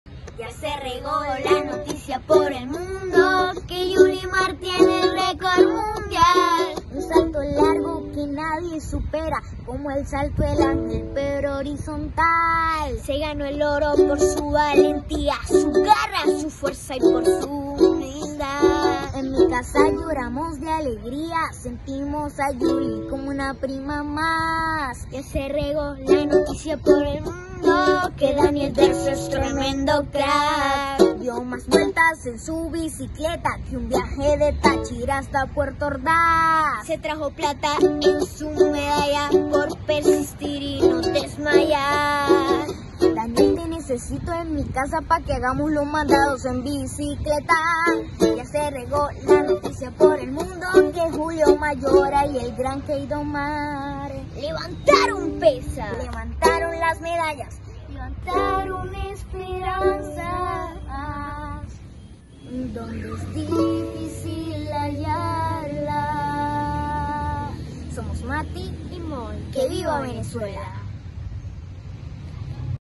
Táchira | Mati y Moi los pequeños tachirenses le dedicaron una canción a medallistas venezolanos
Los niños tachirenses Mati y Moi le dedicaron una canción a la campeona Olímpica, ganadora de medalla de oro, Yulimar Rojas, y a los otros campeones medallistas de Venezuela.